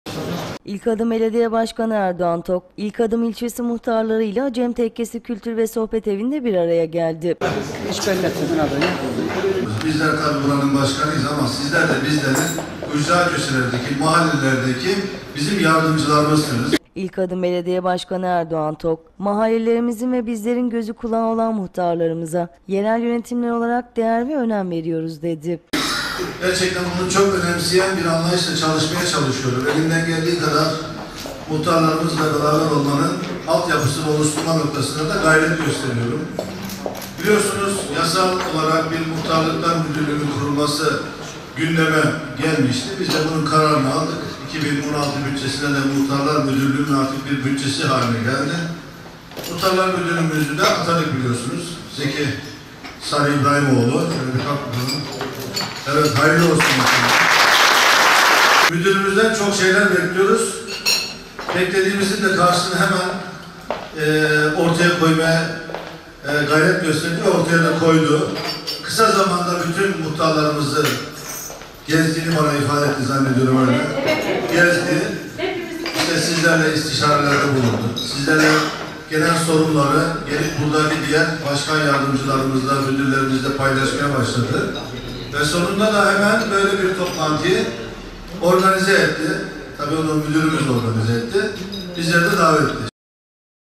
BAŞKAN TOK MUHTARLARLA BULUŞTU
İlkadım Belediye Başkanı Erdoğan Tok İlkadım İlçesi Muhtarları ile Acem Tekkesi Kültür ve Sohbet Evinde bir araya geldi.

     İlkadım Belediye Başkanı Erdoğan Tok ''Mahallelerimizin ve bizlerin gözü kulağı olan muhtarlarımıza yerel yönetimler olarak değer ve önem veriyoruz'' dedi.